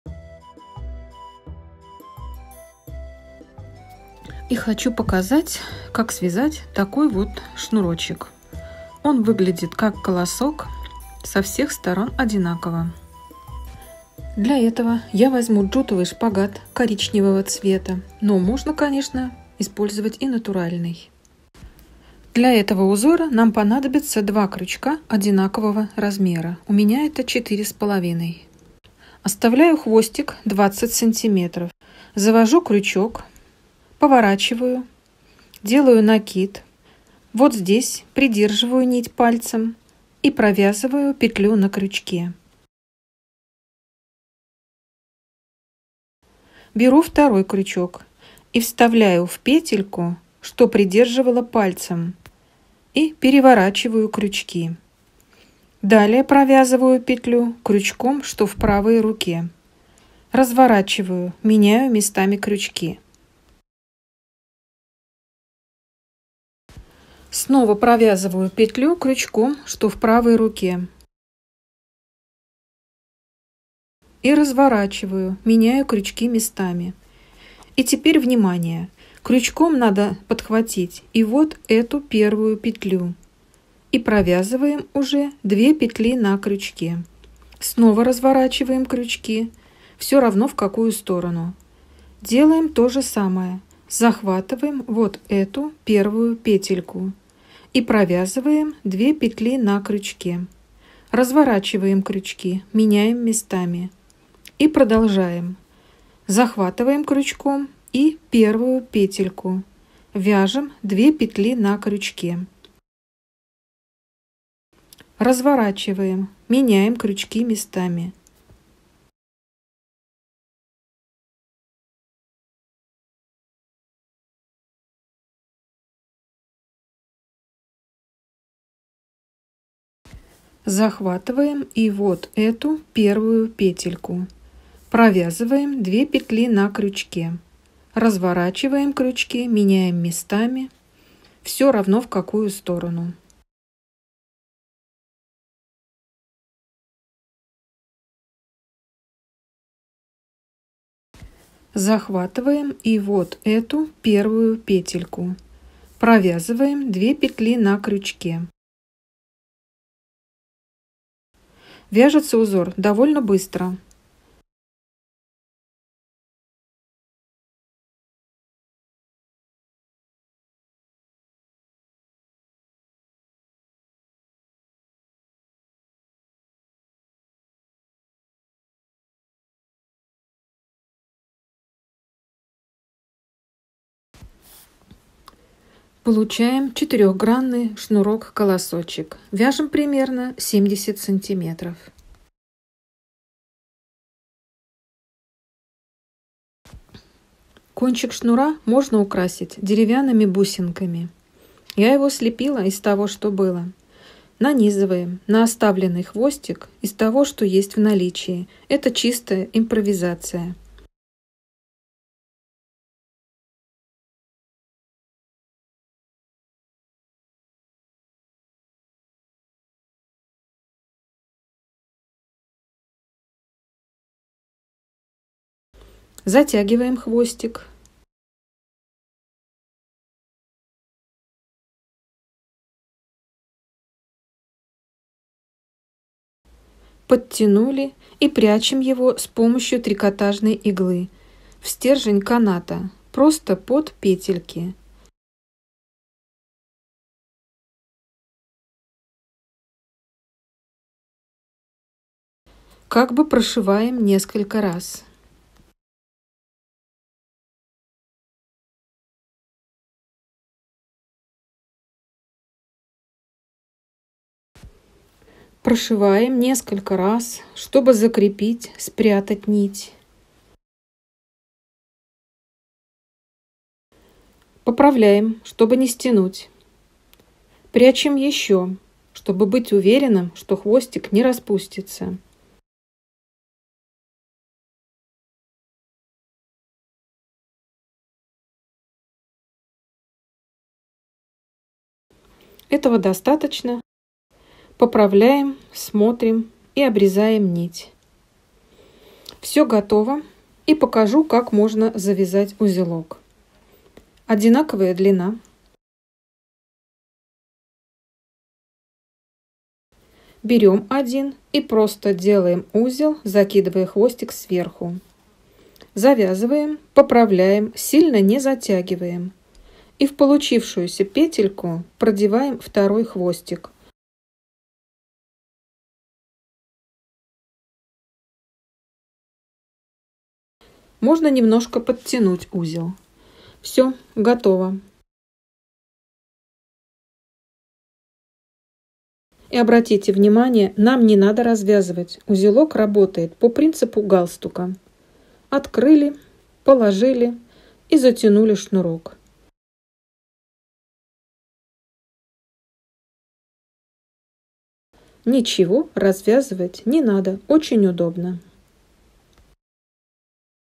ДВА КРЮЧКА и жгутик, шнурок - утяжка ГОТОВЫ !
(ремешок, ремень,  для сумки или рюкзака)
Для вязания этого узора необходимо  ДВА КРЮЧКА !
Этот жгутик - утяжка станет прекрасным дополнением образа!
( альтернатива кожаному )

ВСЕ части Мастер класса по вязанию сумочки "АНАНАС"
смотрите по ссылке ниже